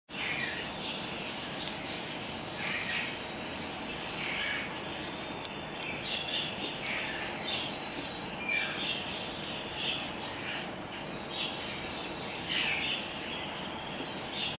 マルギナータリクガメ入荷しました。
ヨーロッパ産のCBベビーです。甲羅の綺麗な優良個体が100匹入荷しています。